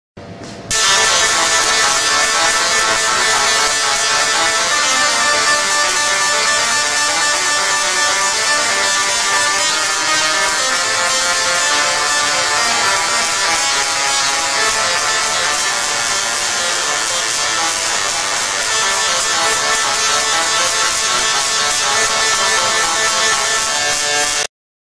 Singing Tesla Coil 2009
A singing Tesla coil designed and built by students at the Department of Electrical and Computer Engineering, University of Canterbury